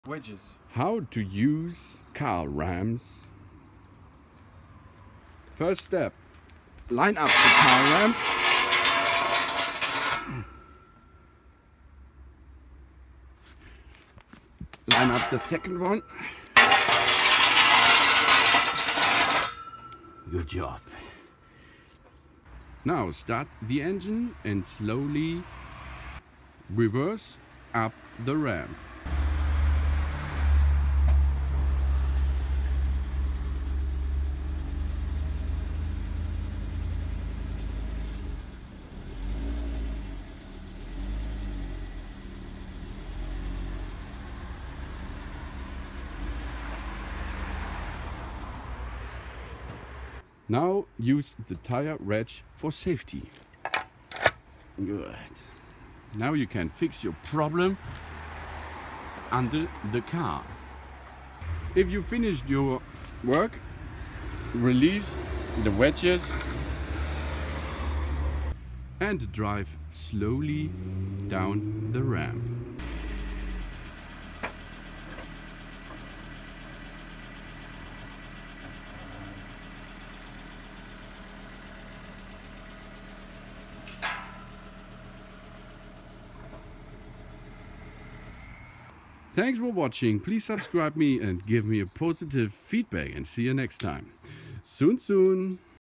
How to use Car Lift Service Ramps Vehicle Ramp rear Version Volvo V70
Slim 11.5 degree loading angle over 66" ramps designed for low profile sports cars and trucks
Lifts a vehicle with low ground clearance 7" off the ground for maintenance and showroom display
16" Wheel platforms with integrated wheel stops
Raised side rails help wheels stay on track during loading and unloading
Made with durable, heavy duty, all-aluminum construction with a 3,000 lb capacity